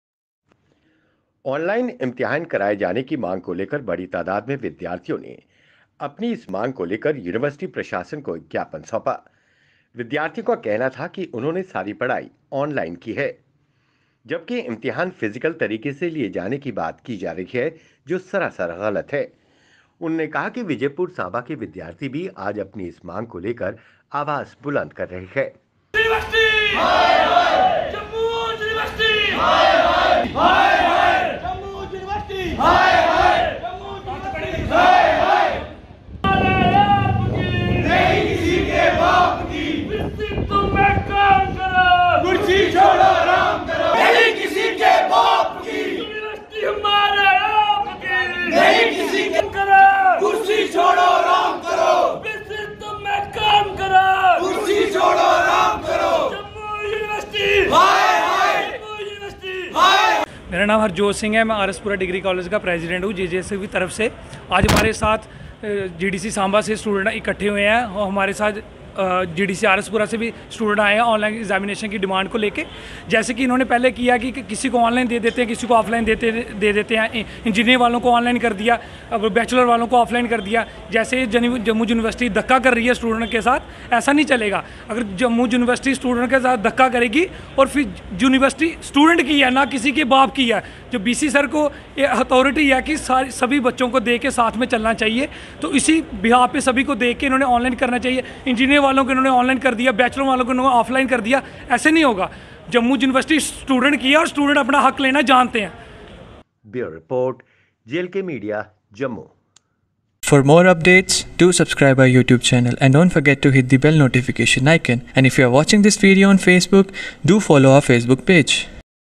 JLKMEDIA Students protests and demands online examination against University administration
Students protests and demands online examination against University administration

（￣ー￣）